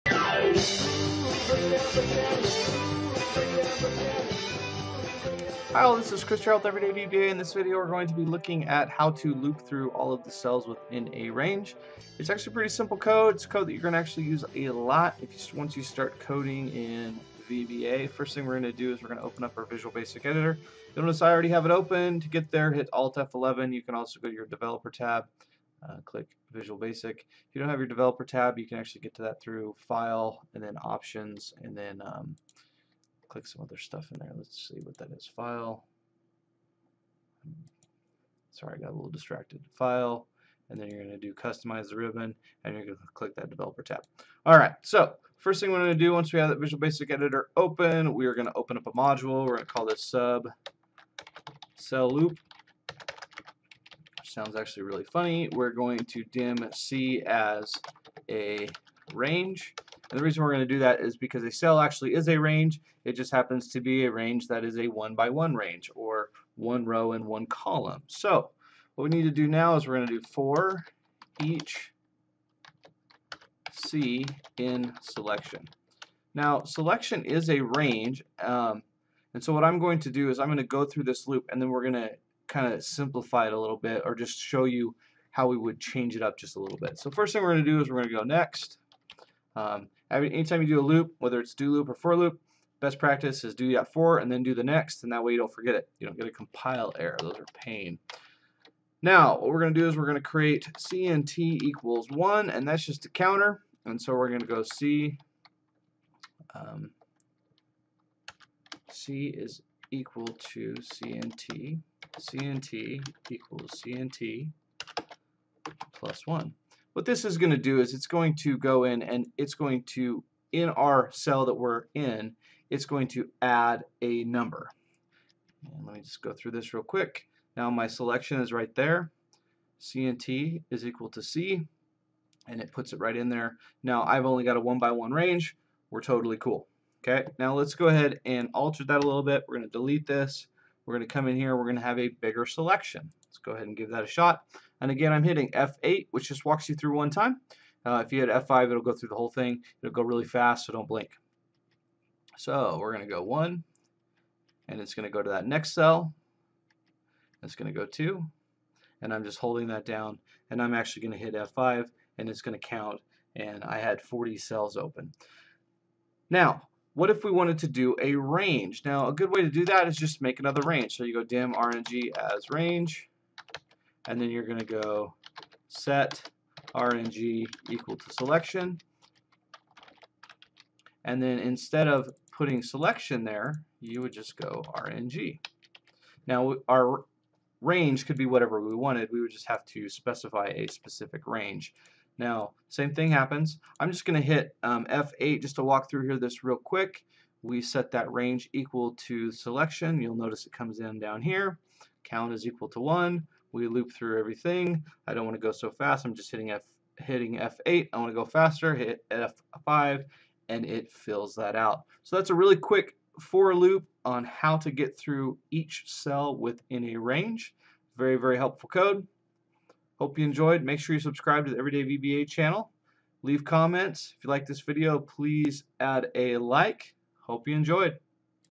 Looping through the Cells in a Range in Excel VBA (Macro) - Code Included
In this video we use a for each loop to loop through all the cells in a range (we used selection because we can).

Code:
Sub CellLoop()

Dim c As Range
Dim rng As Range

Set rng = Selection

cnt = 1

For Each c In rng
    c = cnt
    cnt = cnt + 1
Next

End Sub